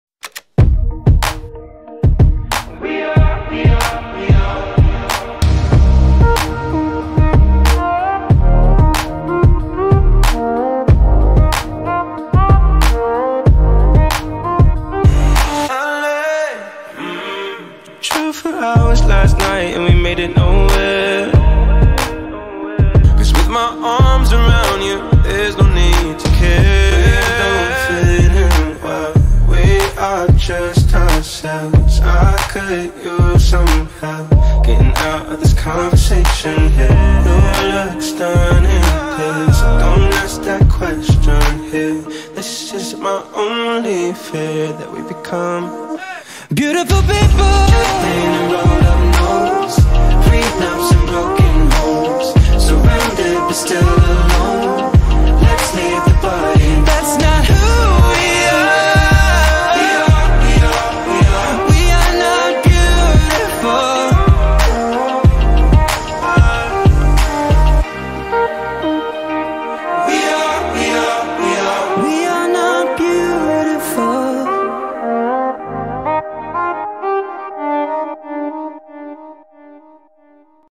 [FMV] JungKook & Jimin (Jikook) - AU Social (Dedicated to Jubsaturn)
How are you? I hope well!

I brought another FMV after a while, from Jikook.

Sorry, but now I'm going to speak in Portuguese!

 @jubsaturn7580 JUJUUUUU.

Um feliz ano novooooo e que esse ano seja muito melhor para a gente. Eu espero que possamos ser amigas, hehe.

Muito obrigada pelos Collabs e MEP que fizemos juntas. Espero que em 2023 possamos voltar com tudo, hahaha.

É isso, espero que goste do FMV, fiz ele com muito carinho!

Artist: Ed Sheeran feat. Khalid